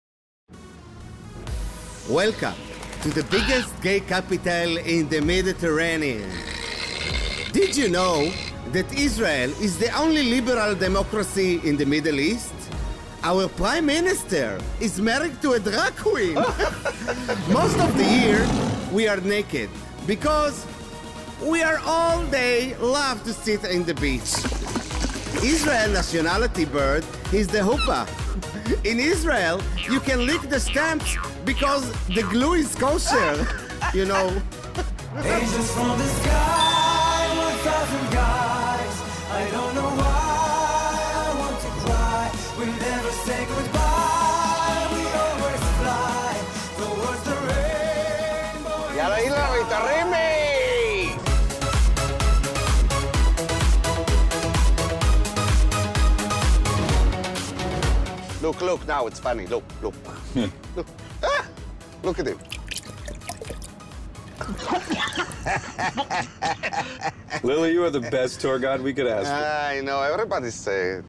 The Bar Mitzvah: Welcome to GAY Israel
Lily's promotional video for Gay Israel.. The Only.. something in the middle east